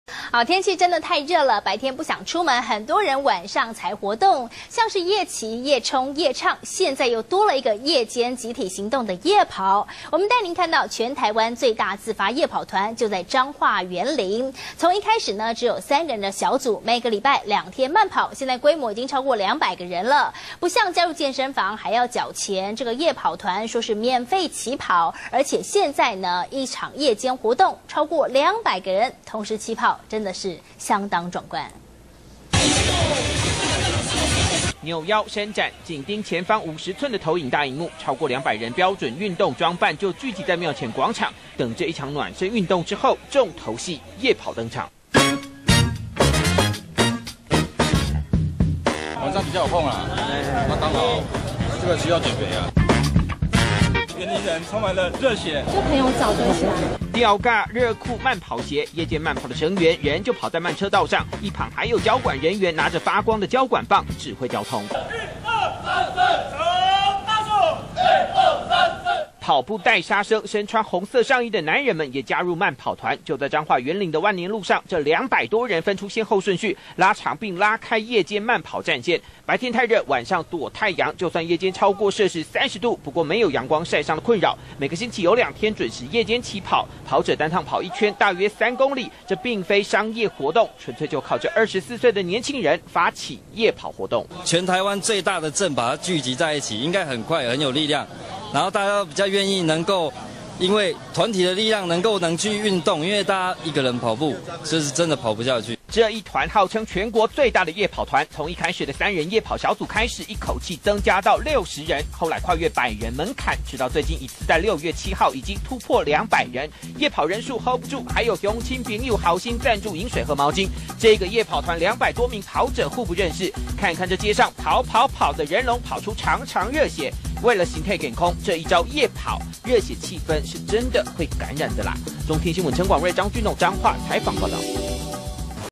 中天新聞》全台最大夜跑團 200人摸黑熱血上路
中天新聞》來看全台灣最大自發夜跑團，就在彰化員林！從一開始的三人夜跑小組，每週兩天慢跑運動，至今規模已超過200人；不像加入健身房要繳交費用，這個夜跑團號稱免費起跑，由於團員慢跑在街道上，已向當地里長報備，還有團員投入交管工作，一場夜間活動，超過200人同時起跑，相當壯觀。